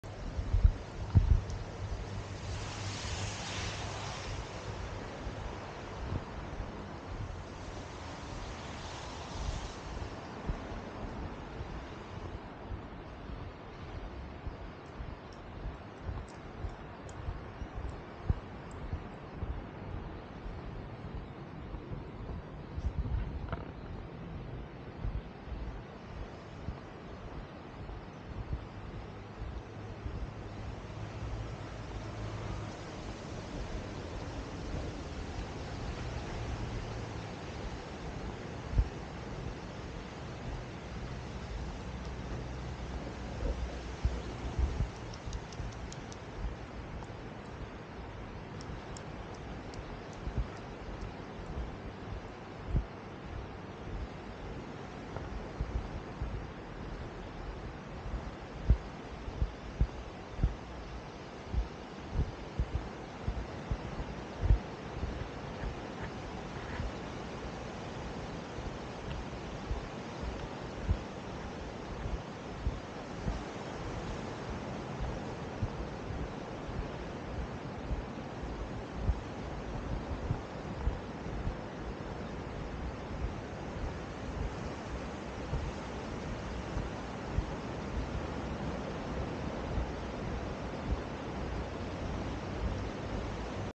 Our Huskies on the beach in VR
This is a 360 VR Video best viewed on a VR headset but can also be viewed on a smart phone by moving the phone around to see in all directions or using the arrow keys on a PC.
Camera: Insta360ONE x2
Initial editing: Insta360 Studio
Final editing: VideoProc Vlogger